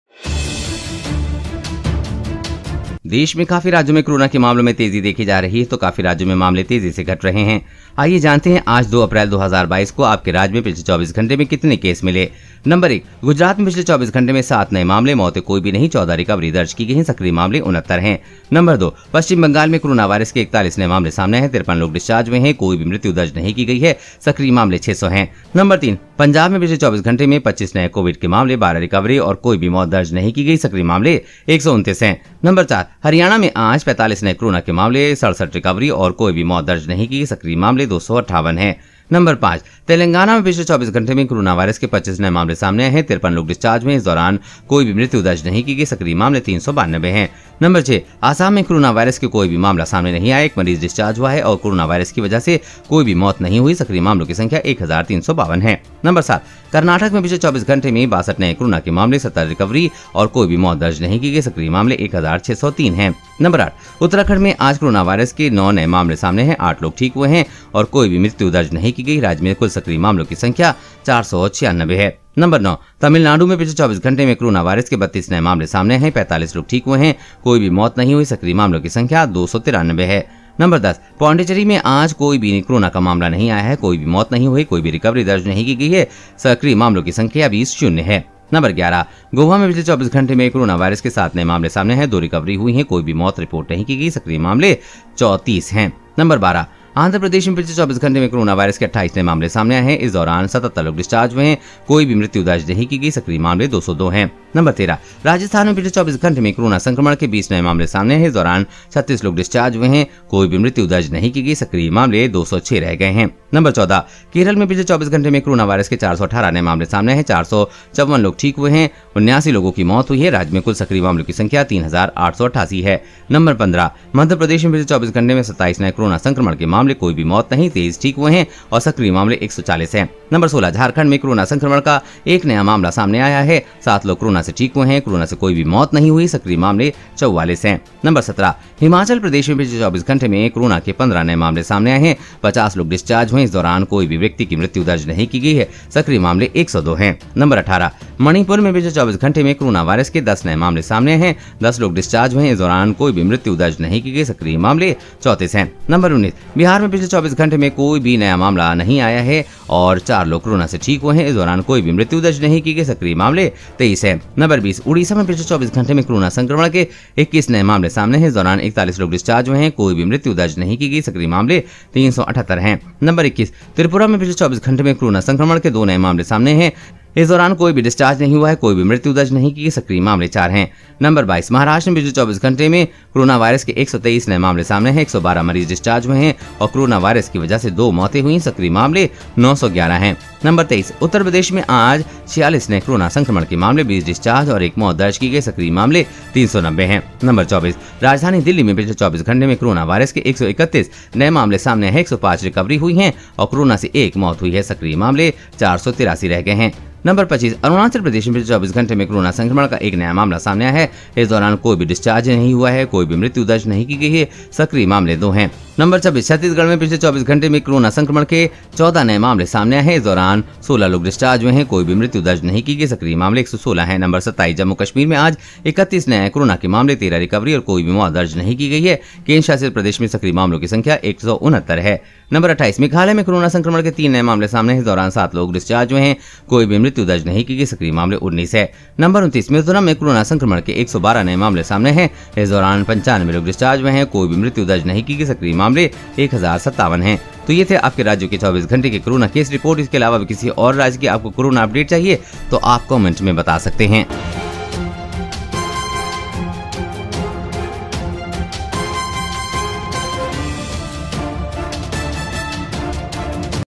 जानिए किस राज्य में कोरोना हुआ आउट ब्रेक ! जानिये सभी राज्यों में कोरोना का हाल ! 02 अप्रैल 2022
Coronavirus (COVID-19) Cases In India State Wise Daily Updates | Coronavirus Cases State Updates | All State Coronavirus Case Update

EDITOR ►Nishant